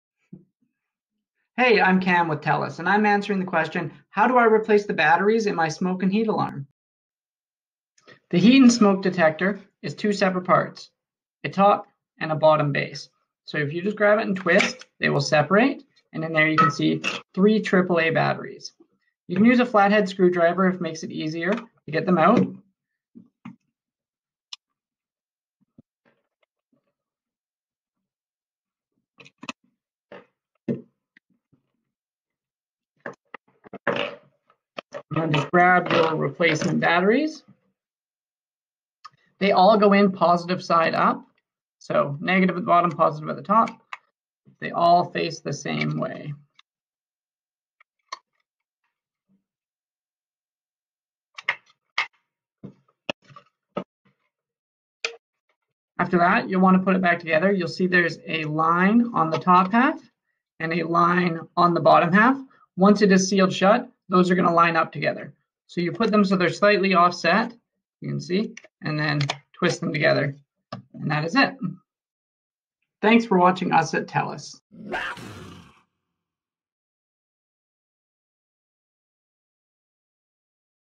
TELUS | How do I replace the battery for my smoke & heat alarm (Gen 2)?
Watch Cameron help answer the frequently asked question (FAQ): "How do I replace the battery for my smoke & heat alarm (Gen 2)?".  Learn how to set up and manage your TELUS SmartHome Security devices and features.  For more information on managing your TELUS SmartHome Security,